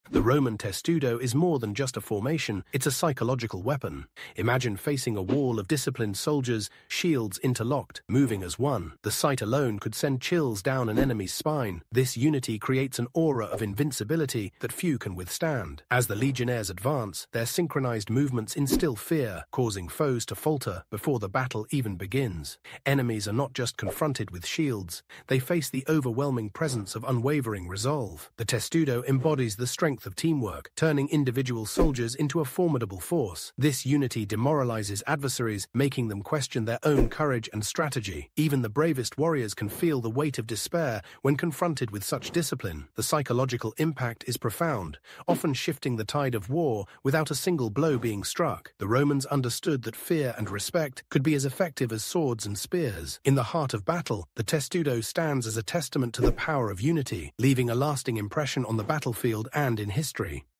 🇮🇹 Roman Testudo Formation!
Ever wondered how the Roman legions dominated the battlefield? In this video, uncover how the ingenious testudo formation became a psychological weapon that struck fear into the hearts of their enemies! From its strategic brilliance to the unity and strength of the Roman soldiers, see why this tactic was a game-changer. 💪🗡️

➡️ Watch Now and Unlock the Mysteries of Ancient Warfare!

Join the adventure and empower your mind with history! 🚀

00:00 Rome
00:00:00.080 the Roman testudo is more than just a
00:00:02.000 formation it's a psychological weapon
00:00:04.319 imagine facing a wall of disciplined
00:00:06.160 soldiers Shields interlocked moving as
00:00:08.559 one the sight alone could send chills
00:00:10.599 down an enemy's spine this Unity creates
00:00:12.920 an aura of invincibility that few can
00:00:15.160 withstand as the legionnaires Advance
00:00:17.279 their synchronized movements instill
00:00:18.960 fear causing foes to falter before the
00:00:21.279 battle even begins enemies are not just
00:00:23.720 confronted with shields they face the
00:00:25.840 overwhelming presence of unwavering
00:00:27.679 resolve the testudo embodies the
00:00:29.599 strength of teamwork turning individual
00:00:31.599 soldiers into a formidable Force this
00:00:33.760 Unity demoralizes adversaries making
00:00:36.000 them question their own courage and
00:00:37.600 strategy even the Bravest Warriors can
00:00:39.600 feel the weight of Despair when
00:00:41.320 confronted with such discipline the
00:00:43.000 psychological impact is profound often
00:00:45.440 Shifting the tide of War without a
00:00:47.320 single blow being struck the Romans
00:00:49.239 understood that fear and respect could
00:00:51.160 be as effective as swords and Spears in
00:00:53.280 the heart of battle the testudo stands
00:00:55.359 as a testament to the power of unity
00:00:57.280 leaving a lasting impression on the
00:00:58.840 battlefield and in history